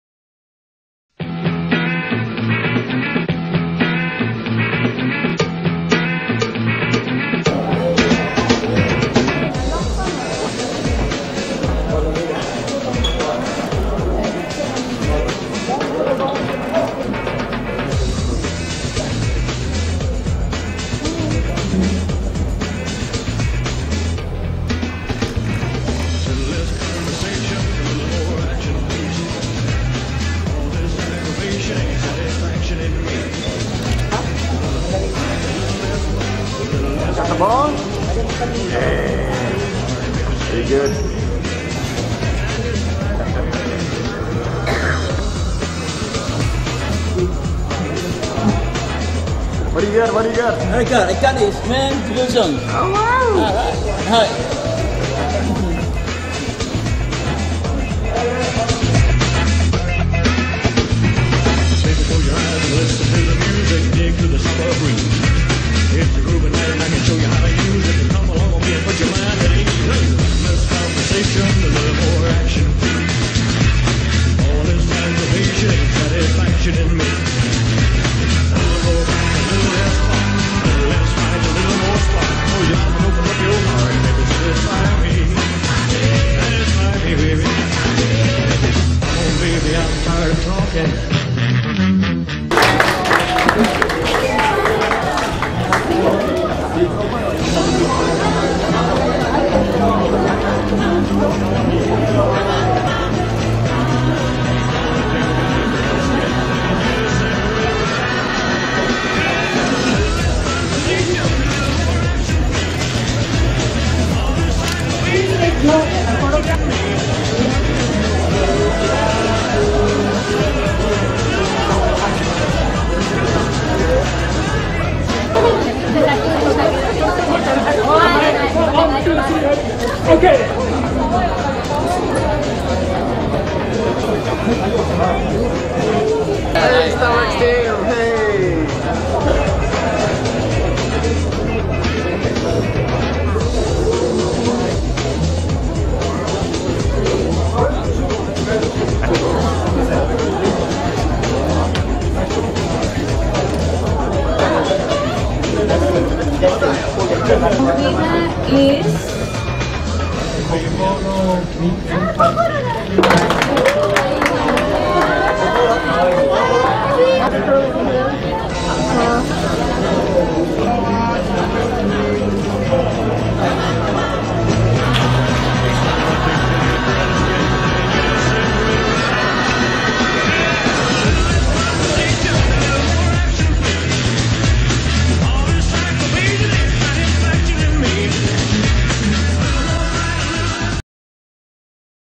ATP World Tour 250 Malaysian Open 2010 - Video Of The Day - Day 2 - Part 2 - 26th Sept 2010
These are a series of daily videos from the ATP 250 Malaysian Open held at Stadium Putra, Bukit Jalil in Kuala Lumpur,the capital of Malaysia . The videos highlighted the interesting activities happening throughout the event. It covers behind the scenes, the audiences, players at the tournament.